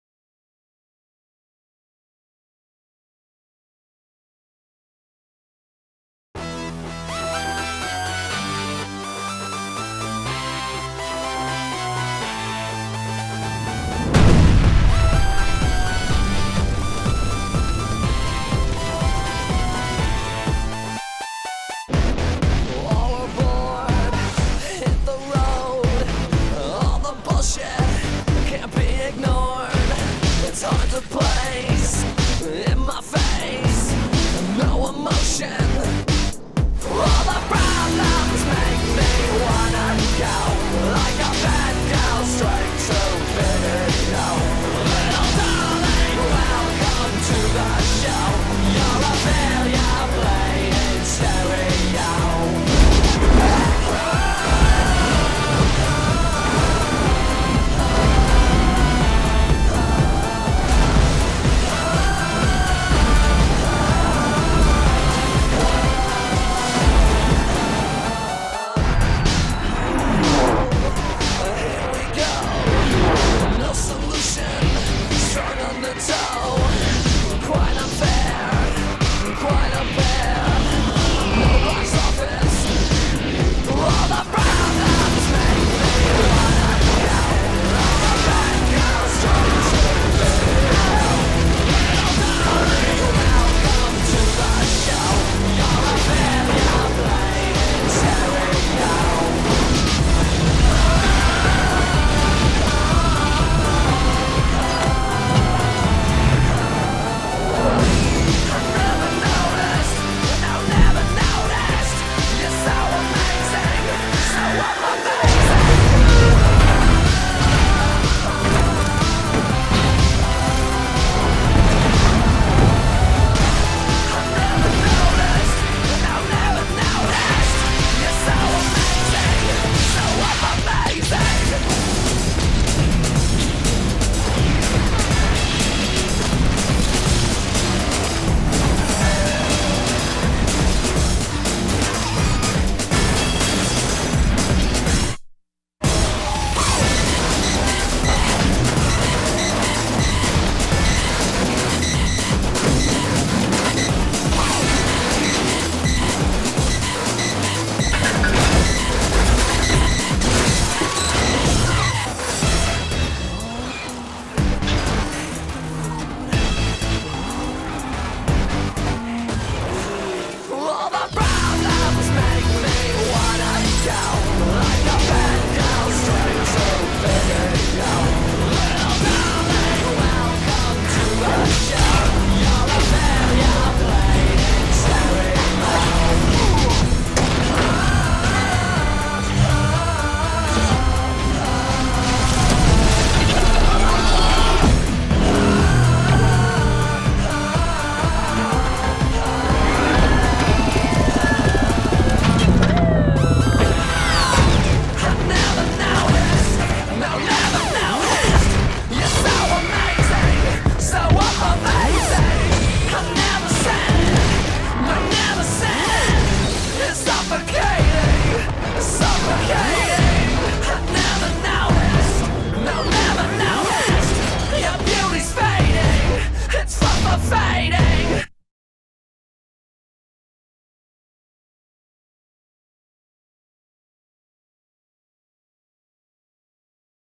MICHAEL BAY is EPIC! (Full HD)
Showcasing explosive and epic action from Michael Bay movies.

A remake of the video by Decimus13 from 2006.  Most of the video is edited to the original clips frame by frame.  Enjoy!